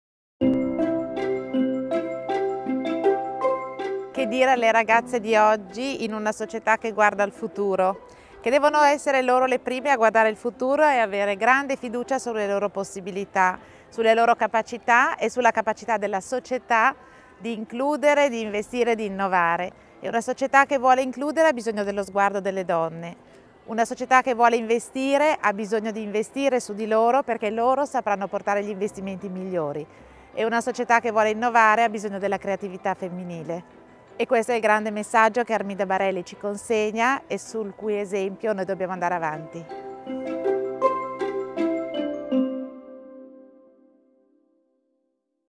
98a Giornata per l'Università Cattolica - Antonella Occhino
Le ragazze devono per prime guardare al futuro e avere fiducia nelle proprie capacità. Perché una società che vuole includere e innovare ha bisogno della creatività femminile. Si rivolge proprio alle giovani studentesse Antonella Occhino, preside della facoltà di Economia all'Università Cattolica di Milano.